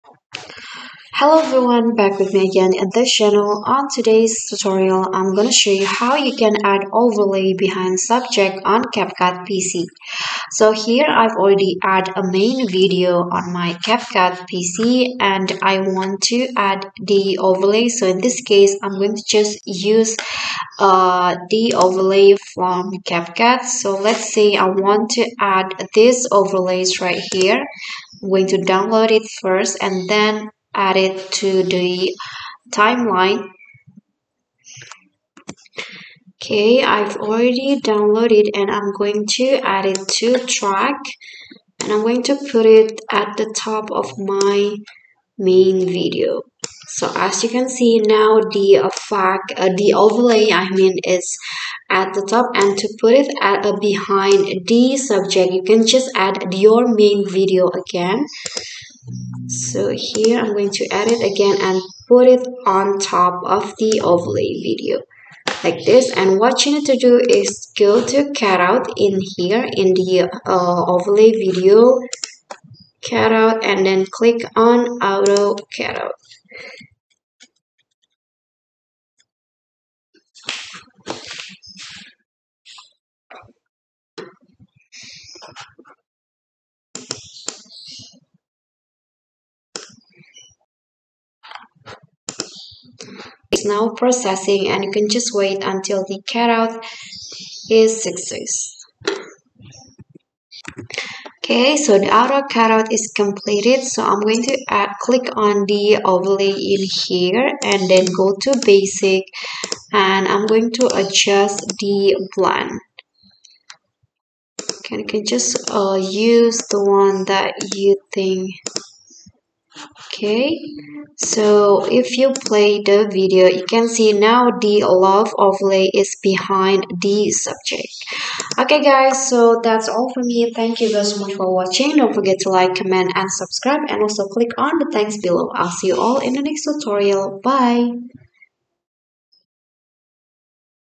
Easy! How To Add Overlay Behind Subject On CapCut PC UPDATED
Here's how to put overlay behind the subject of a video on CapCut PC